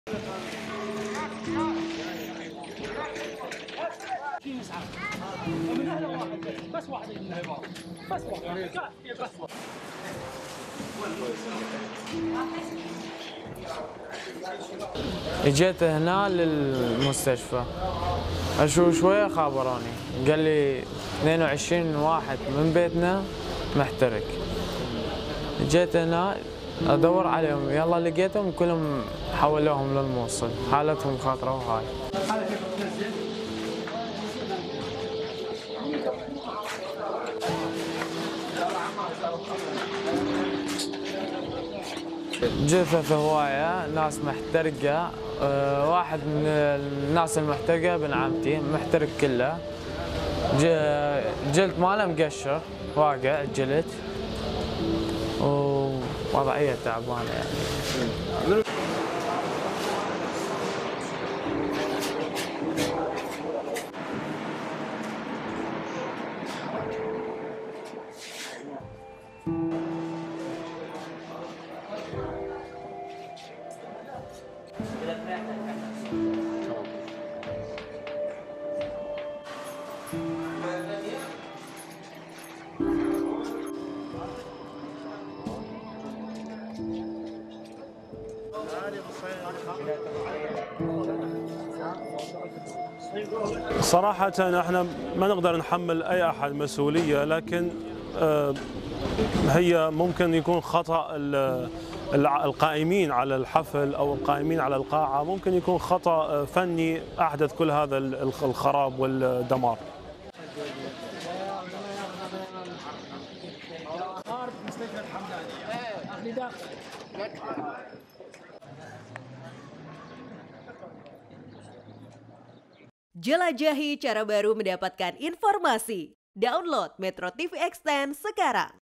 200 Orang Terluka, Korban Kebakaran Hamdaniya Dilarikan Ke Rumah Sakit
MetroTV, Sedikitnya 114 orang t3w4s dan sekitar 200 lainnya luka-luka dalam kebakaran di sebuah pesta pernikahan di provinsi Nineveh, Irak utara, pada Selasa, kata Gubernur Najim al-Jabouri.